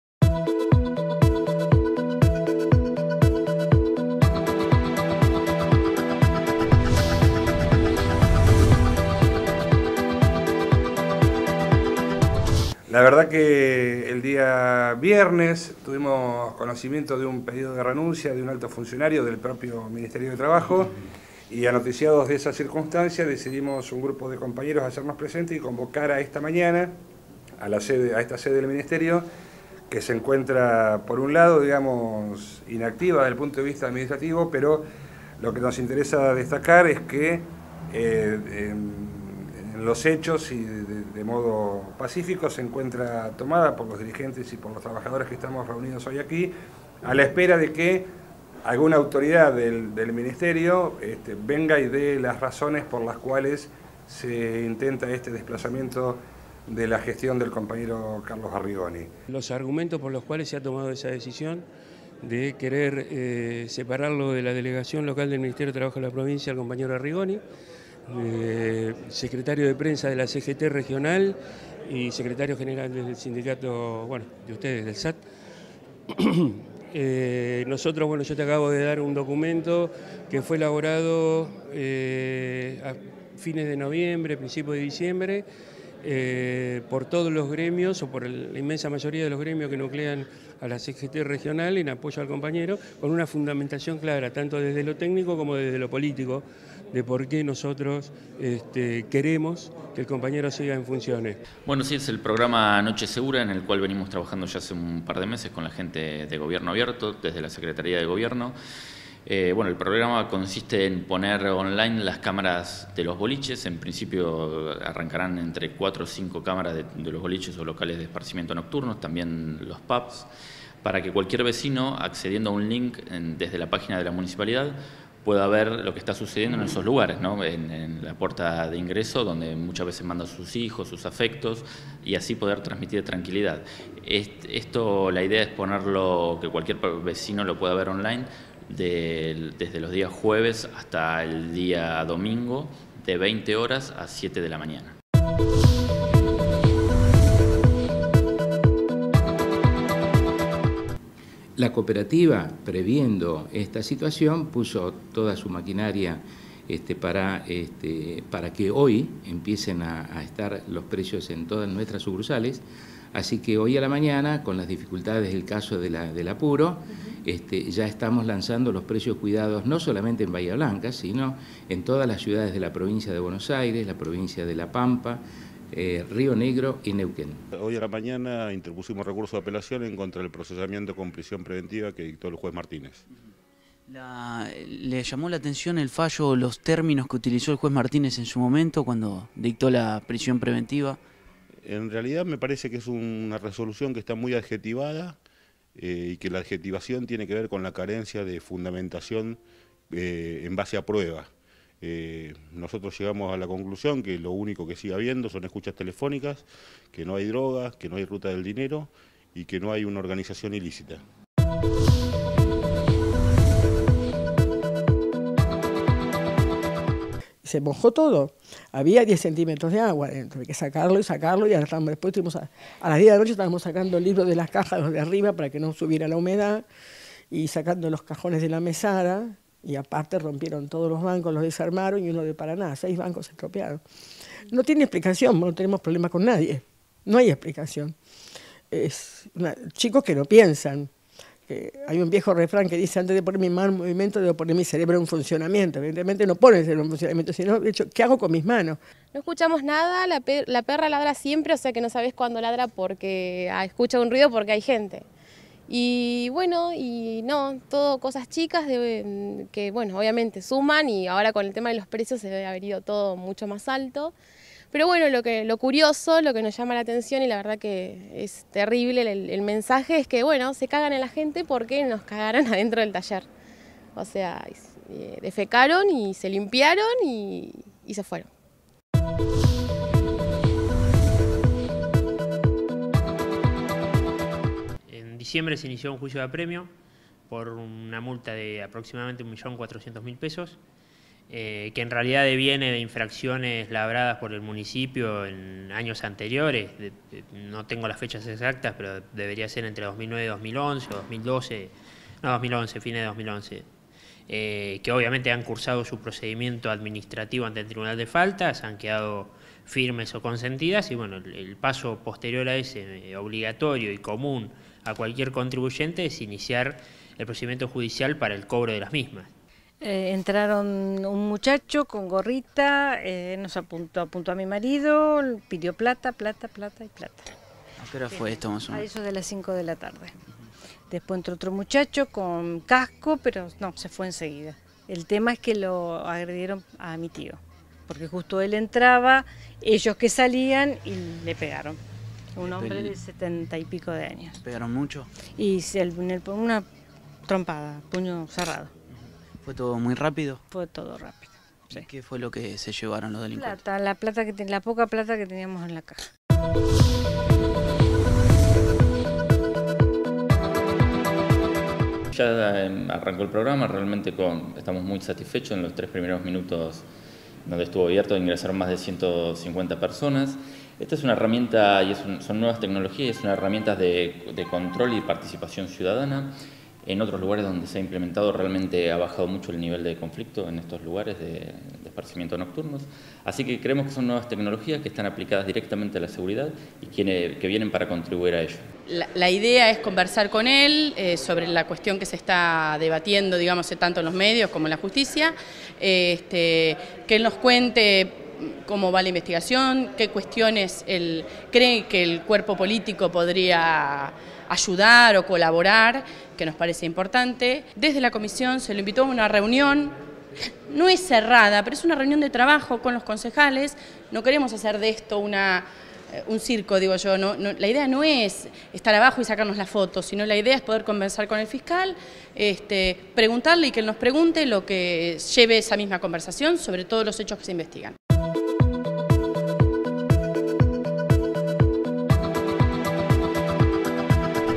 LA SEMANA EN FRASES 7 DE FEBRERO 2014
Compartimos los hechos destacados de ésta última semana. La causa narco, los hechos de inseguridad, la toma en el ministerio de trabajo, y las cámaras de seguridad en zona de boliches son algunos de  los más importantes.